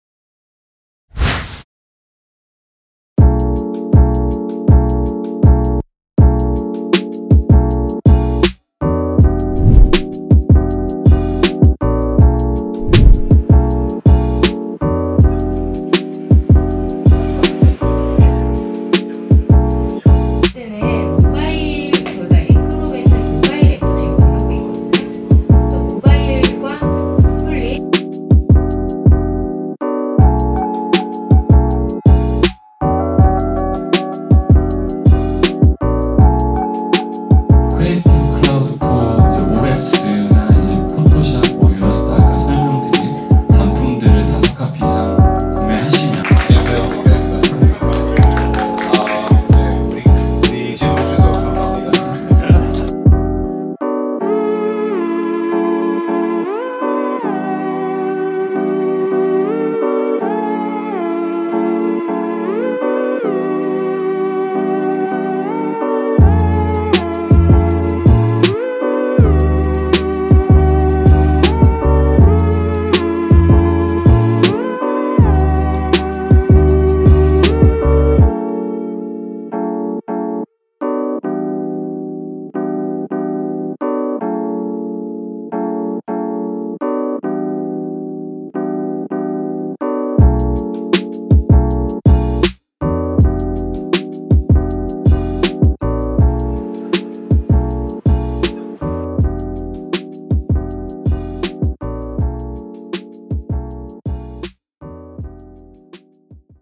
어도비 세미나 in 이태원 레스토랑 젤렌 (feat.미카엘 셰프)
이태원 레스토랑 젤렌에서 열린 세미나
색보정을 위해 s-log로 첫 촬영을 해봤는데
색보정은 무지어렵네.. 좀더 연습해보아야겠다